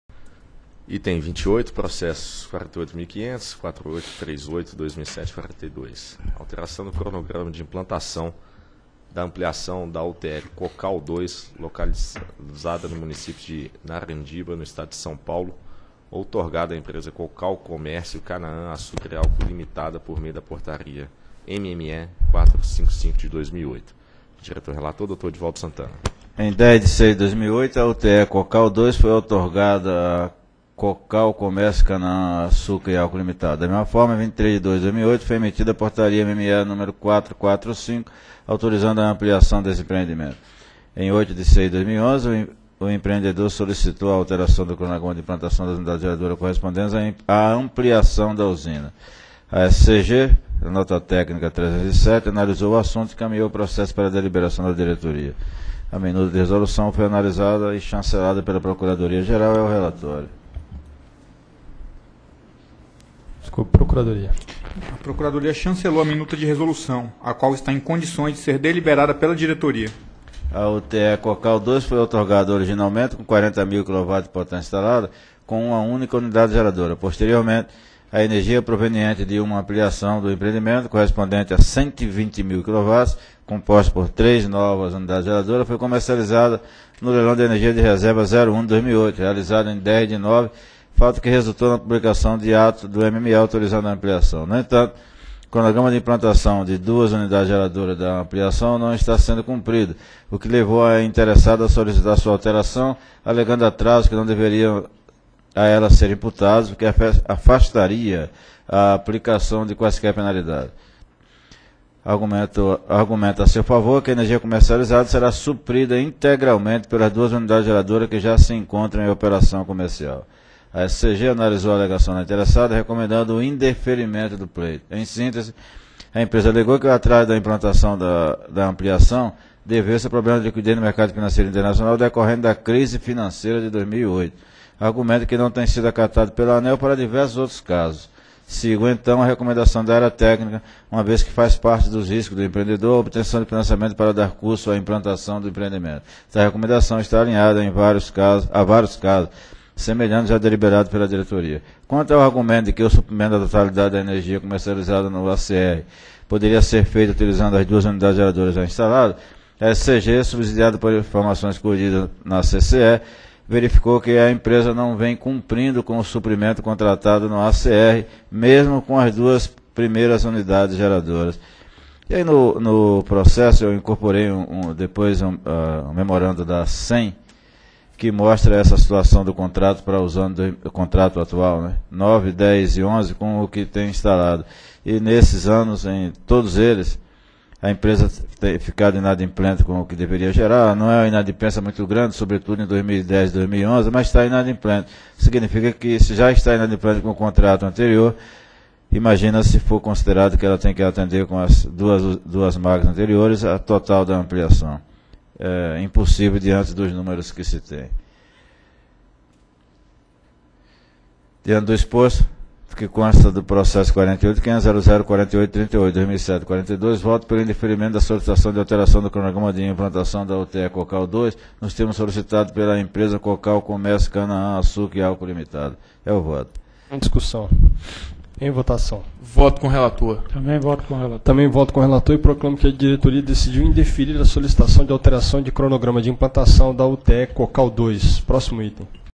Item 28 - 33ª Reunião Pública Ordinária ANEEL 04/09/2012
Item 28- Processo nº: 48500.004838/2007-42. Assunto: Alteração do cronograma de implantação da ampliação da Usina Termelétrica -- UTE Cocal II, localizada no município de Narandiba, no estado de São Paulo, outorgada à empresa Cocal Comércio Canaã Açúcar e Álcool Ltda., por meio da Portaria MME nº 455/2008. Relator: Diretor Edvaldo Alves de Santana.